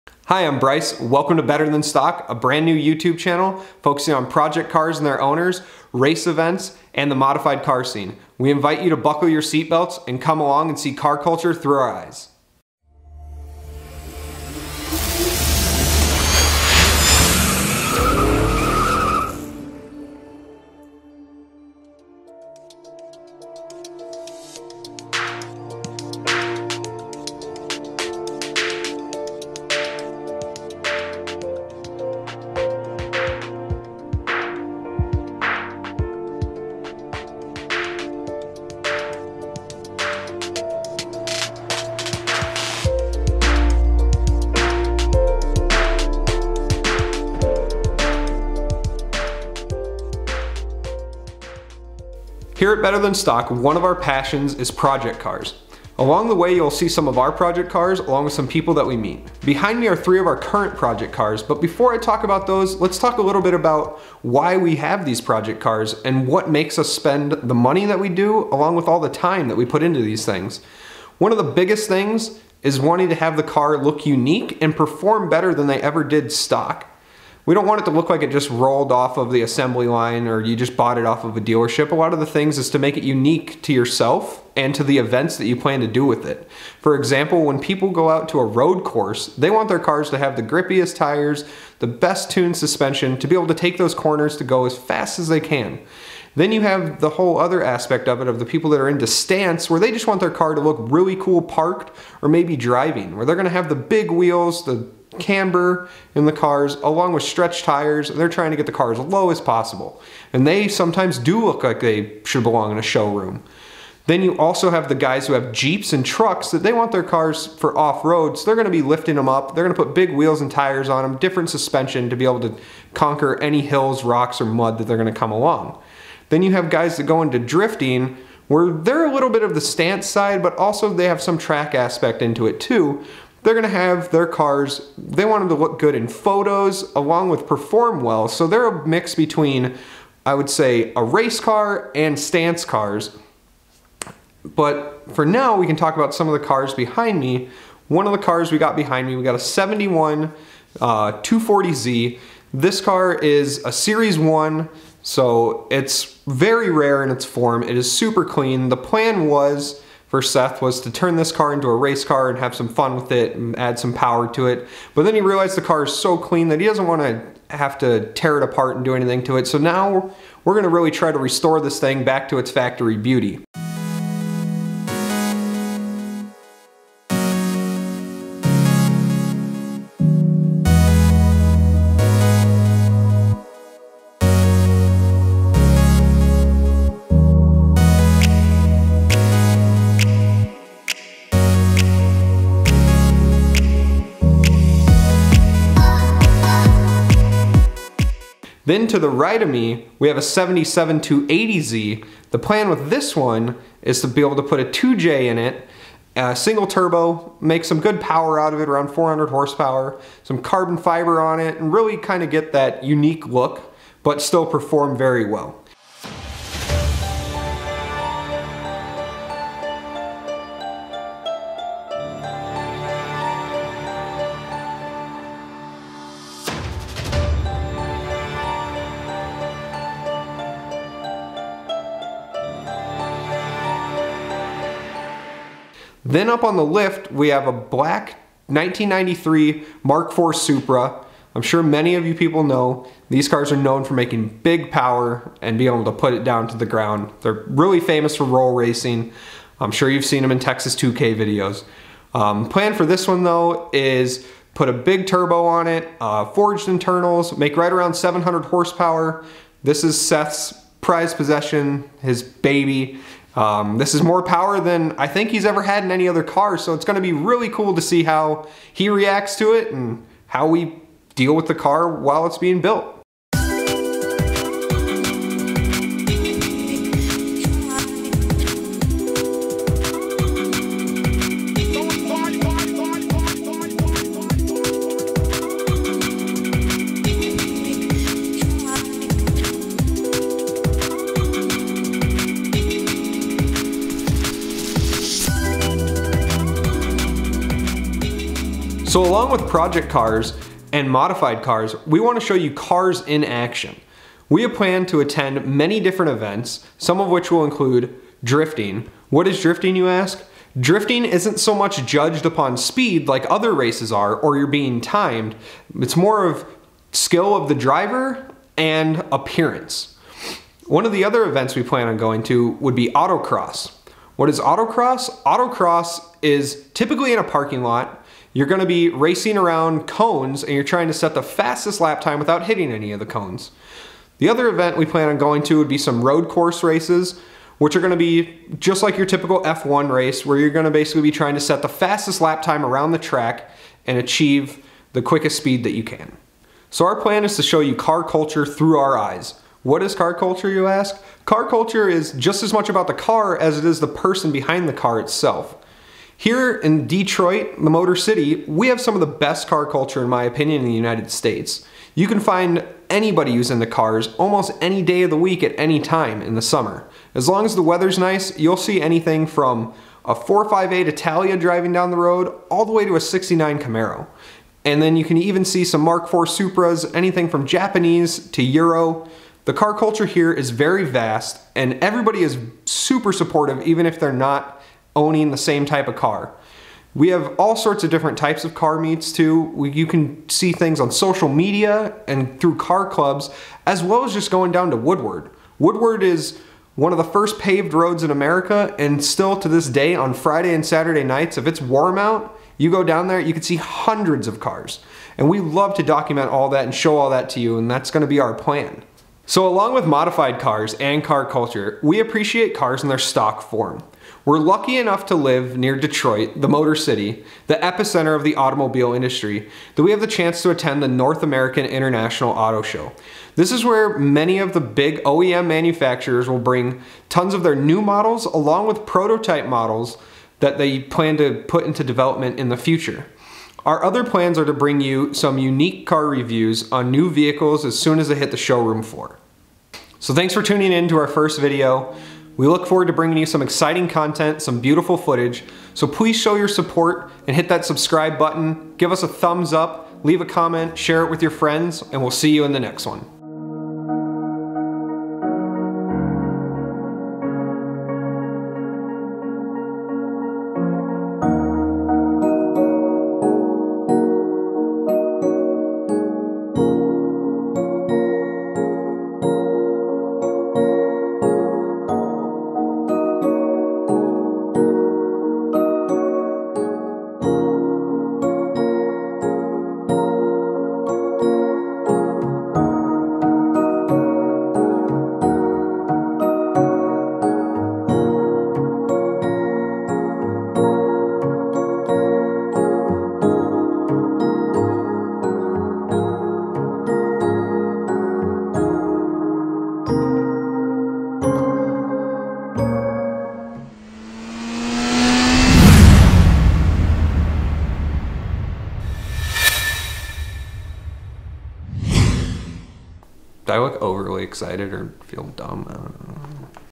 Intro to BetterThanStock (4k)
Our first video!  In this video Bryce explains why we are interested in cars, what project cars we are working on, and what events we plan to document in the future.  This is harder than it looks, so give us some time and we will get better with our video quality, speaking on camera and editing video.